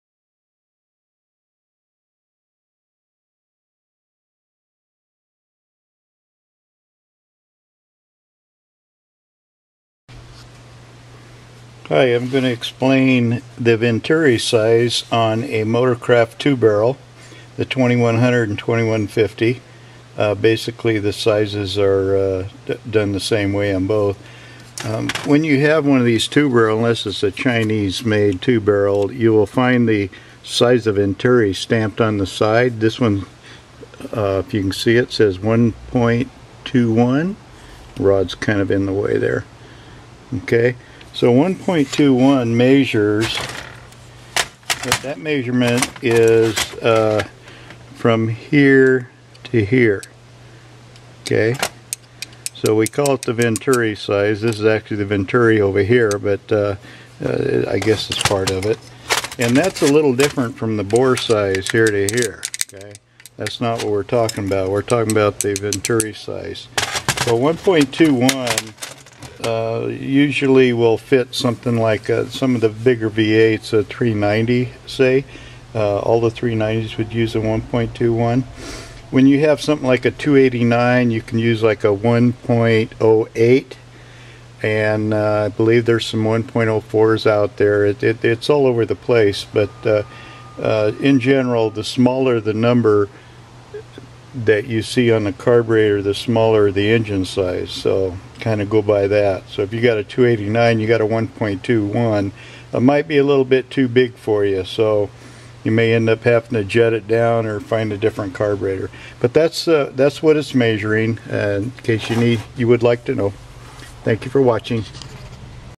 Motorcraft 2 Barrel Carburetor Venturi Size
In this enlightening video, we explore the significance of venturi size in Motorcraft 2-barrel carburetors. Venturi size plays a critical role in determining the airflow and fuel mixture delivered to the engine, ultimately influencing its performance and efficiency.

Join us as we delve into the intricacies of venturi sizing, examining how different sizes affect engine performance under varying conditions. Whether you're a seasoned mechanic or a curious car enthusiast, this video provides valuable insights into optimizing your engine's airflow dynamics.

With clear explanations and visual demonstrations, we uncover the impact of venturi size on fuel atomization, throttle response, and overall engine power. Don't miss out on this opportunity to deepen your understanding of carburetor tuning—watch now and discover the importance of Motorcraft 2-barrel carburetor venturi sizes!